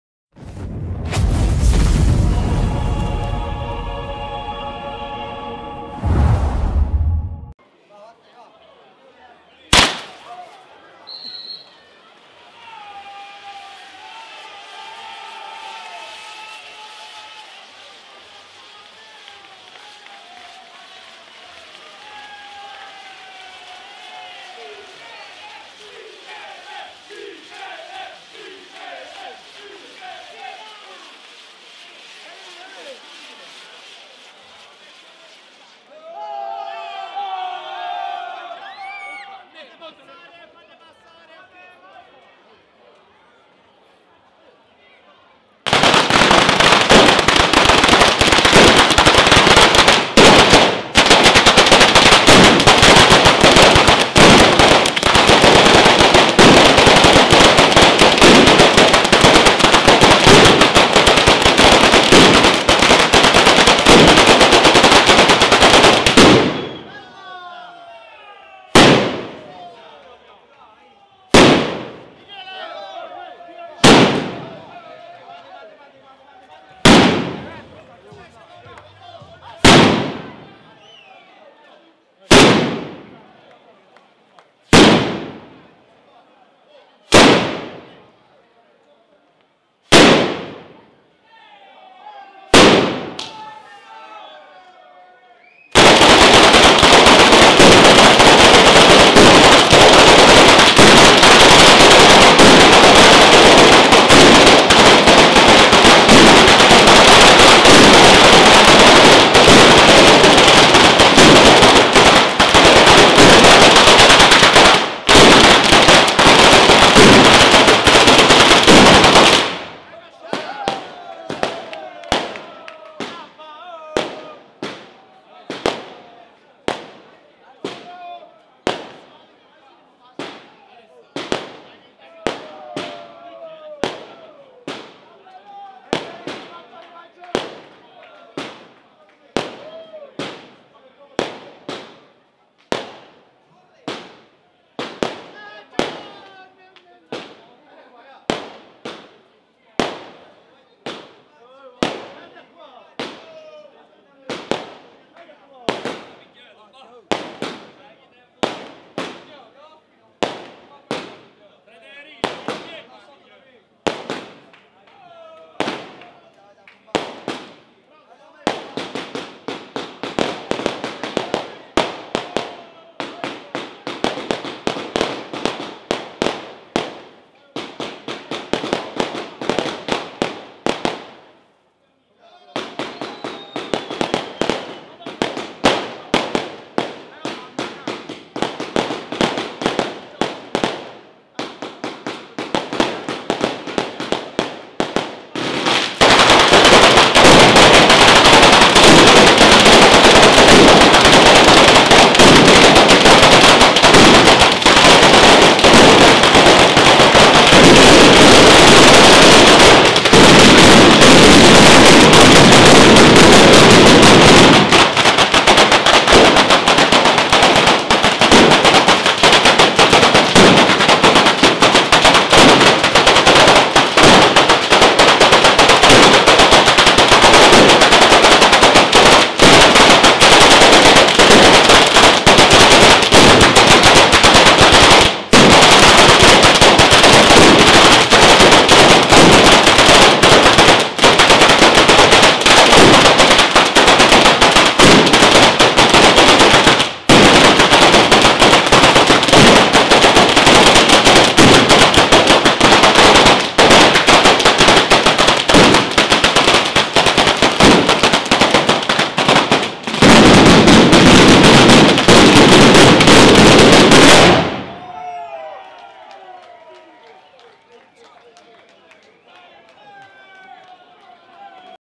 Bellona (Ce) 2019 - Sesta batteria Pirotecnica Chiarappa
Sesta batteria circuito pirotecnico a cura della Ditta Pirotecnica Chiarappa da San Severo (Fg)